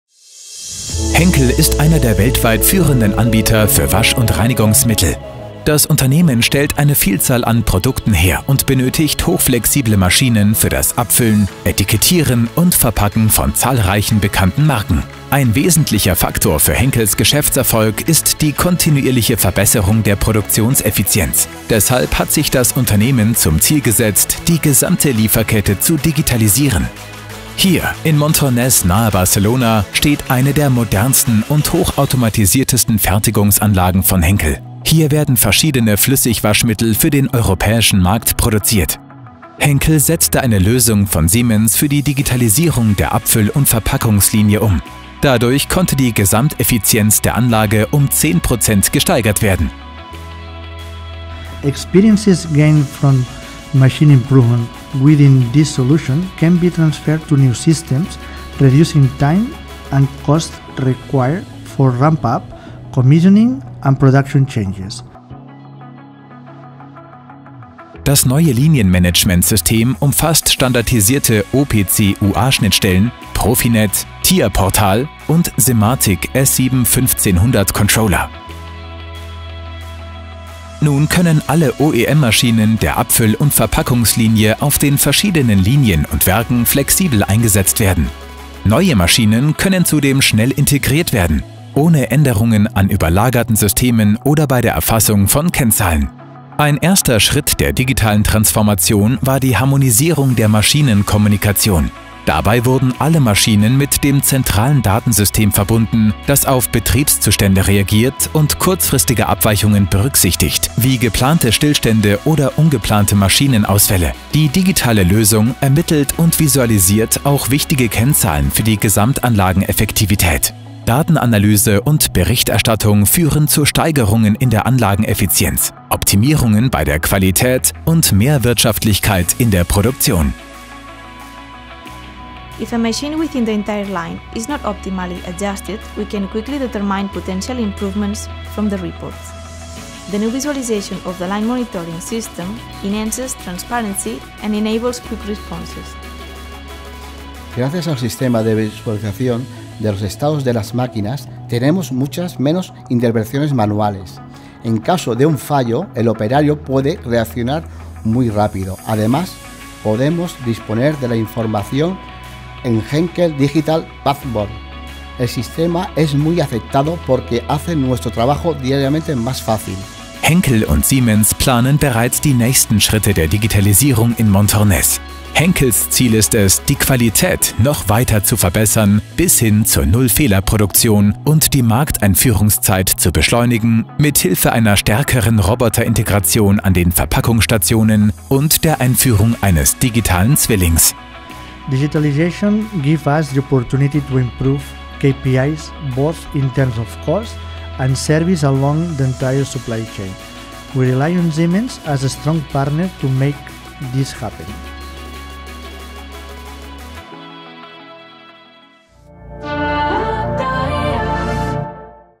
Making efficiency happen - Henkel steigert Effizienz der Produktionslinie um zehn Prozent
Durch die Hardware, die Software und das Know-How konnten wir Henkel dabei helfen die Effizienz seiner Produktion, durch die Digitalisierung, um zehn Prozent zu steigern.
Sie wollen auch von über 170 Jahren Erfahrung profitieren? Treten Sie mit uns in Kontakt! Unter:

Siemens.